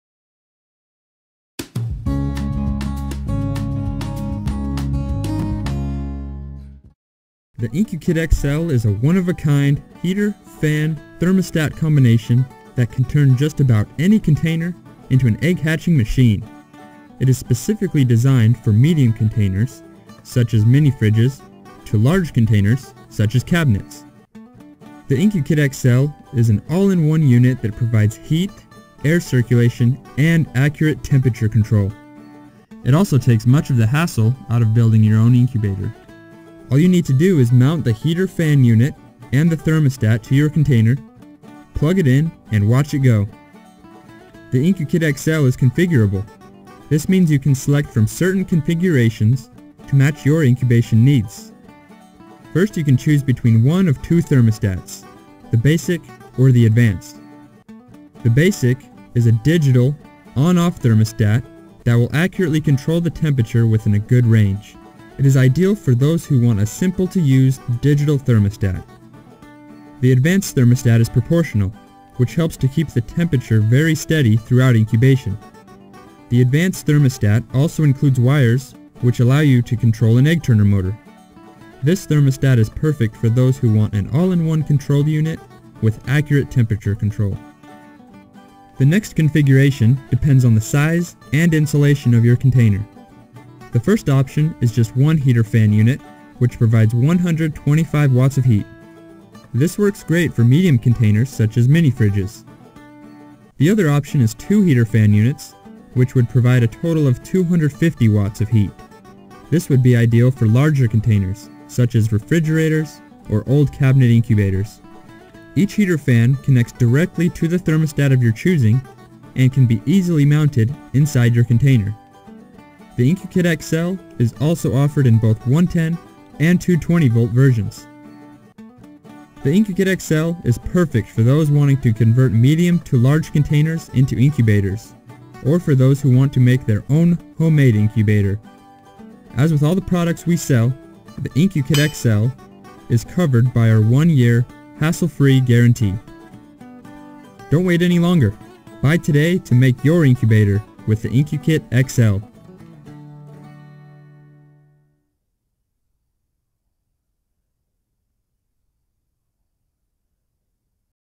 IncuKit XL Product Video
The IncuKit XL is a one-of-a-kind heater, fan thermostat combination that can turn just about any container into an egg-hatching machine. It is specifically designed for medium containers such as mini fridges to large containers such as cabinets.

Features/Applications:
The IncuKit XL is an all-in-one unit that provides heat, air circulation, and accurate temperature control.  It also takes much of the hassle out of building your own incubator. All you need to do is mount the heater/fan unit and the thermostat to your container, plug it in and watch it go! The IncuKit XL is configurable. This means you can select from certain configurations to match your incubation needs. First you can choose between 1 of 2 thermostats: The Basic or the Advanced. The Basic is a digital on/off thermostat that will accurately control the temperature within a good range. It is ideal for those who want a simple-to-use digital thermostat.  The Advanced thermostat is proportional, which helps to keep the temperature very steady throughout incubation.  The Advanced thermostat also includes wires which allow you to control an egg turner motor. This thermostat is perfect for those who want an all-in-one control unit with accurate temperature control.
The next configuration depends on the size and insulation of your container. The first option is just one heater/fan unit which provides 125 watts of heat. This works great for medium containers such as mini fridges. The other option is 2 heater/fan units which would provide a total of 250 watts of heat. This would be ideal for larger containers such as refrigerators or old cabinet incubators. Each heater/fan connects directly to the Thermostat of your choosing and can be easily mounted inside your container. The IncuKit XL is also offered in both 110 and 220 Volt versions.

The IncuKit XL is perfect for those wanting to convert medium to large containers into incubators, or for those who want to make their own home made incubator.
As with all the products we sell the IncuKit XL is covered by our one year hassle-free guarantee.